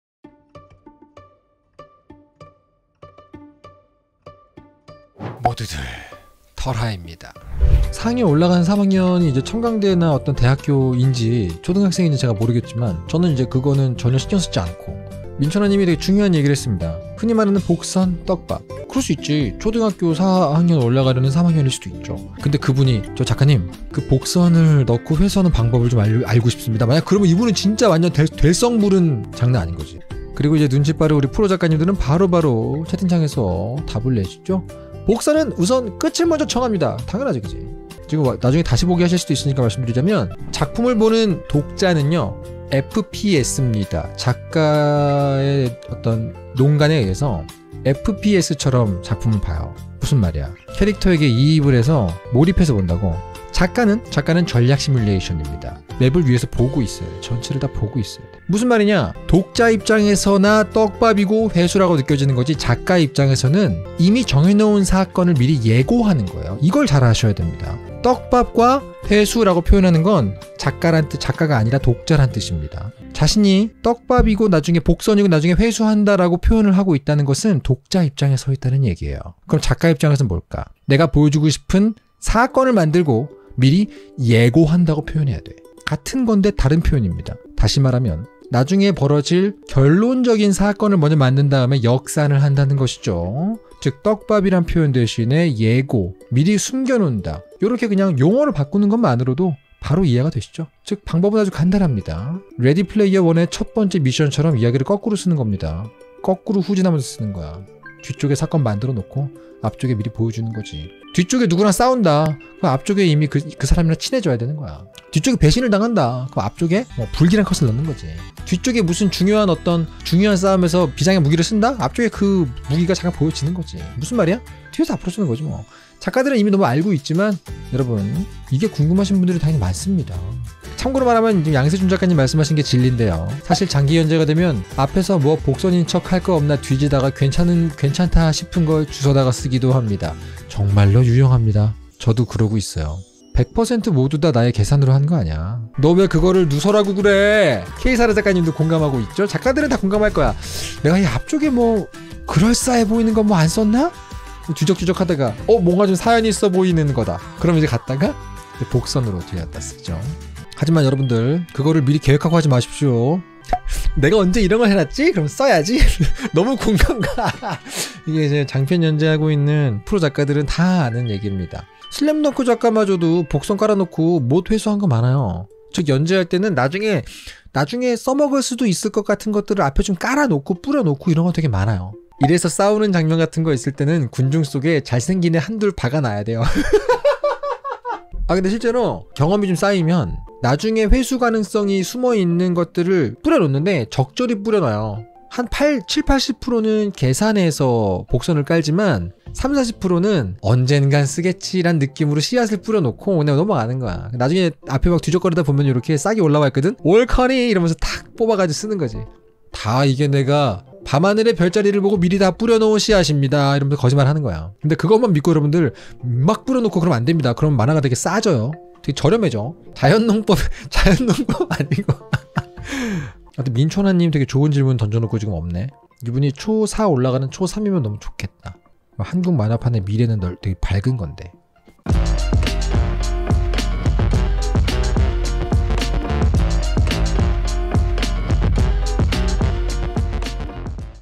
🙋🏻‍♀️ : 선생님! 웹툰에서 복선(떡밥)은 어떻게 회수하나요?
본 영상은 2월 1일에 진행된 라이브 영상 편집본입니다.